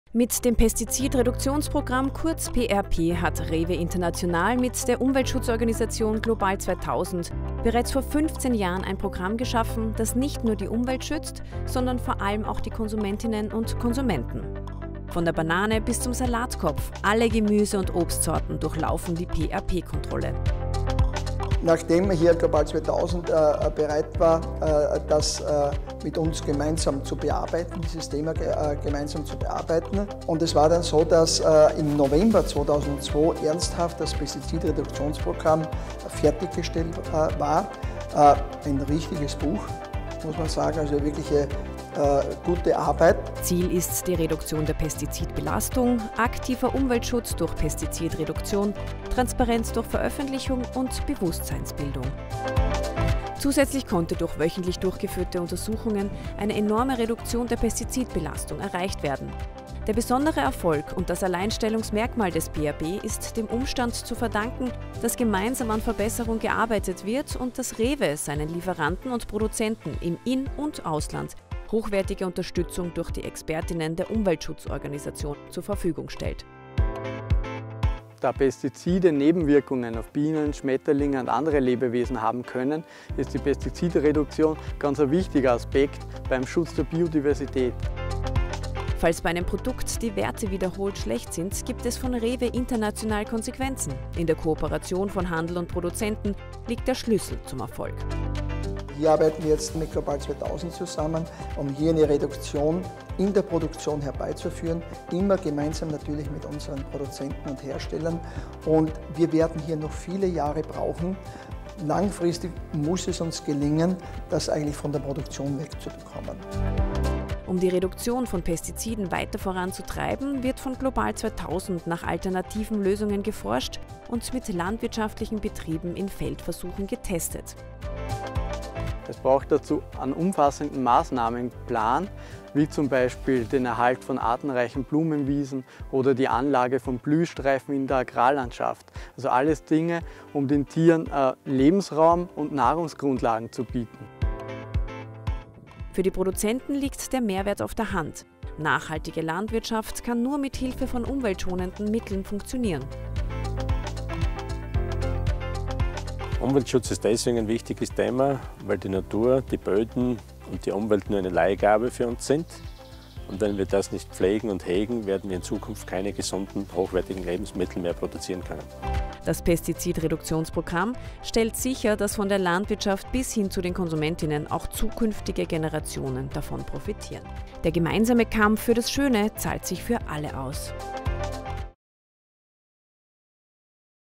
GLOBAL 2000 PestizidReduktionsProgramm
Seit mittlerweile 15 Jahren kooperieren GLOBAL 2000 und REWE zur Pestizidreduktion bei Obst und Gemüse. 2002 testeten wir bei mehreren Handelsketten Paprika aus Spanien auf Pestizid-Rückstande. Die Ergebnisse waren alarmierend: Auf dem an sich gesunden und beliebten Gemüse fanden sich hohe Belastungen mit gesundheitsschädlichen Pestiziden. GLOBAL 2000 forderte, dass die Produktion zunehmend auf Pestizide verzichten und der Lebensmittel-Einzelhandel Verantwortung für die Gesundheit von KonsumentInnen und ProduzentInnen wahrnehmen muss. BILLA reagierte und schlug eine Kooperation vor: GLOBAL 2000 sollte Lösungen für das Problem entwickeln, und so fiel 2003 der Startschuss für das PestizidReduktionsProgramm (PRP).